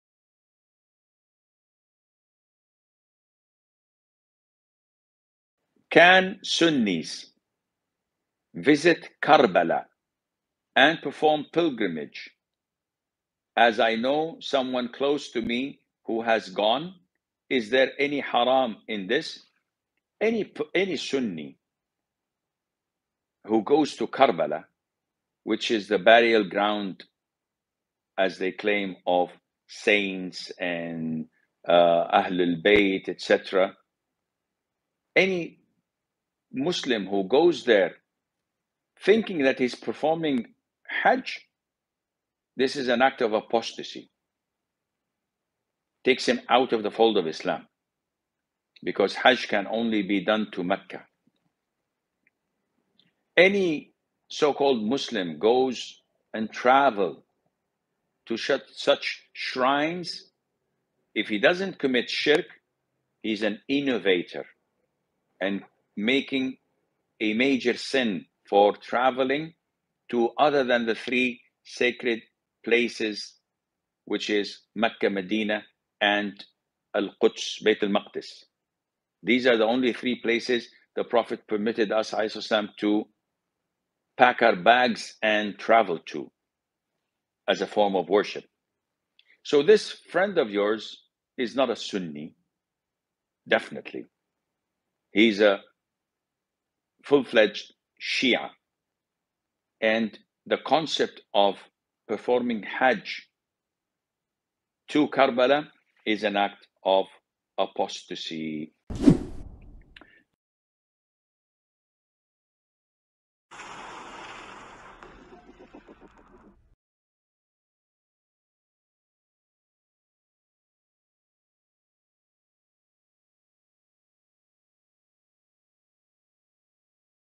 Can Sunni's visit Karbala and perform pilgrimage there? assim al hakeem JAL
00:00 Intro & Ques
00:19 #1 Scenario
00:52 #2 Scenario